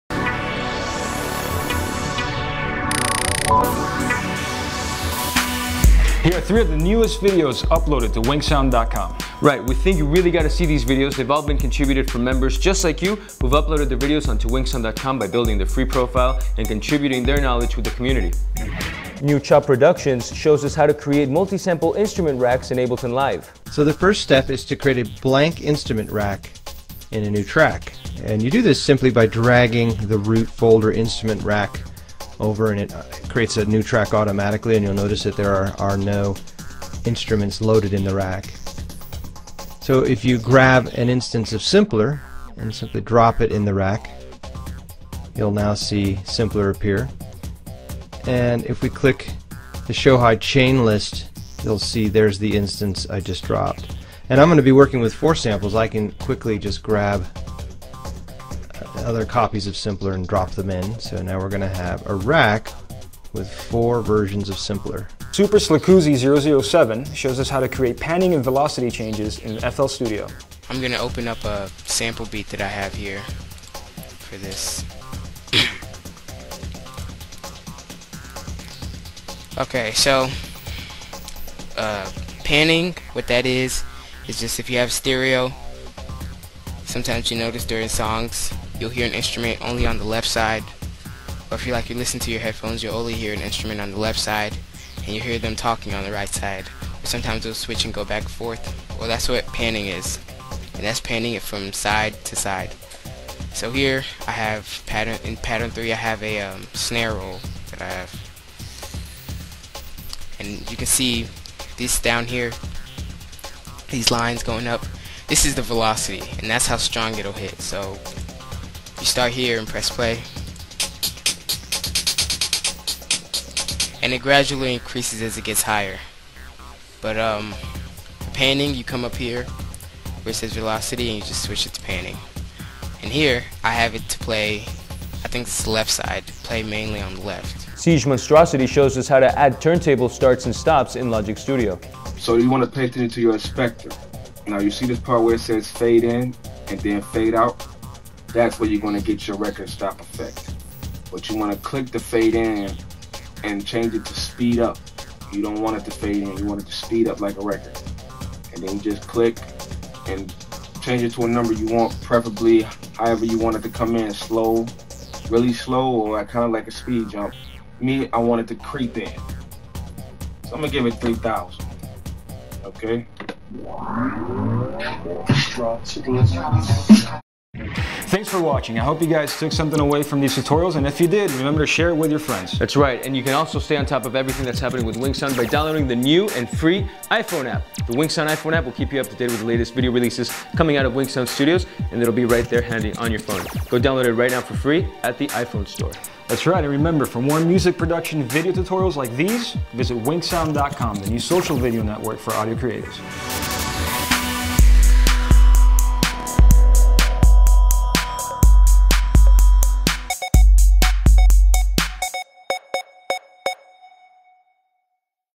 WinkSound.com: Must Watch 23
Watch the latest and greatest videos created by our users and uploaded to the WinkSound community. In this session Mark Mosher of newchoproductions shows you how to create multi-samples instrument racks within Ableton Live, Superslikuzi007 explains how to create panning and velocity changes in FL Studio and Seigemonstracity teaches you how to add turntable style start and stops within Logic Studio.